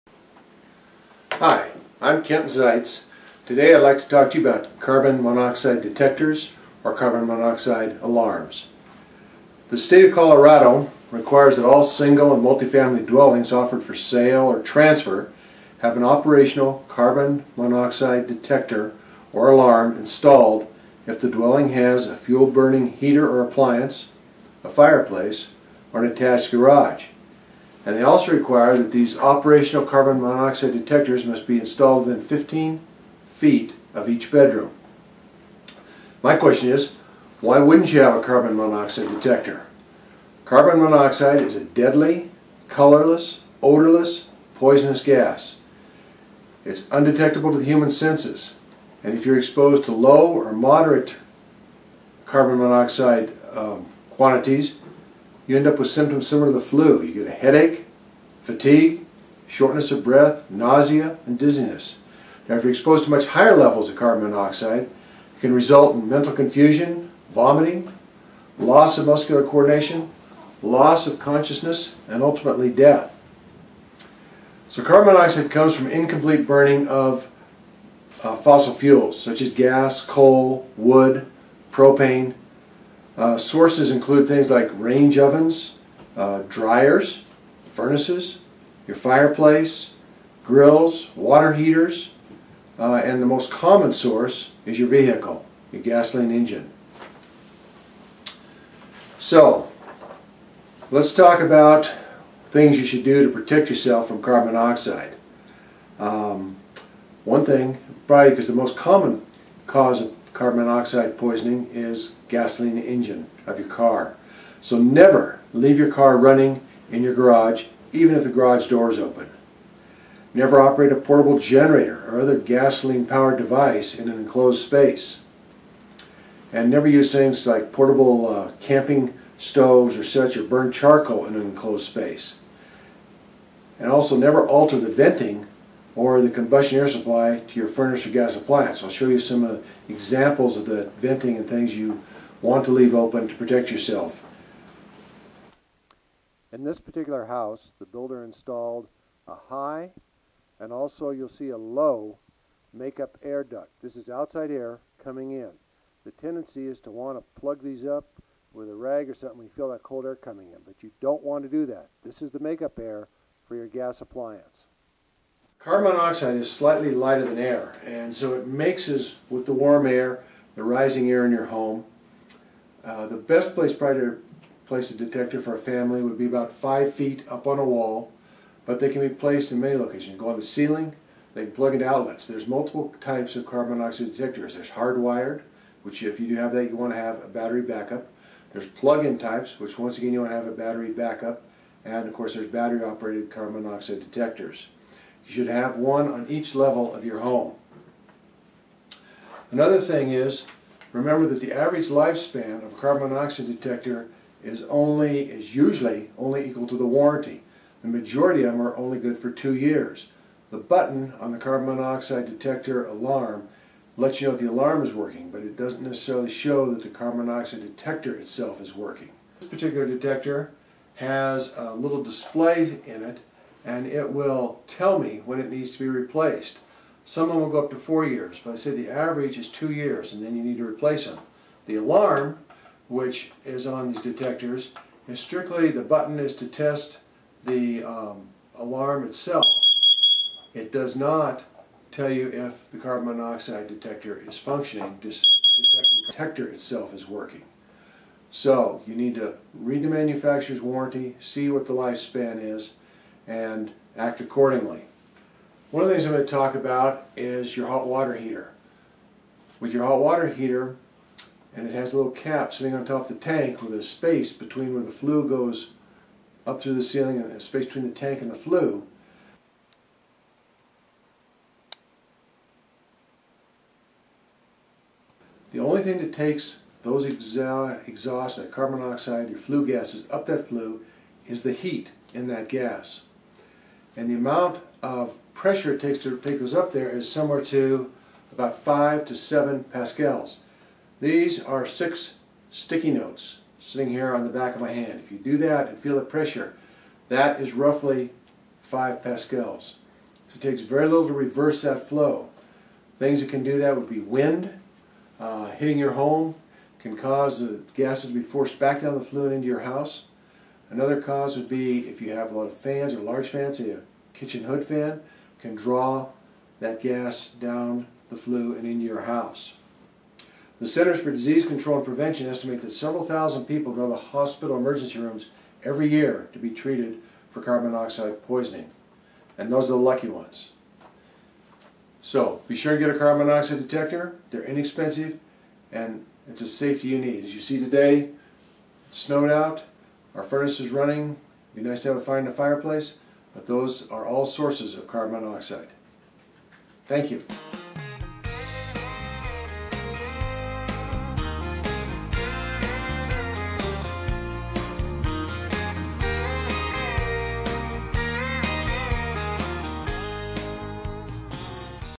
Importance of Having a Carbon Monoxide Detector at Home
Learn the dangers that Carbon Monoxide can cause and the importance of having a carbon monoxide detector at home.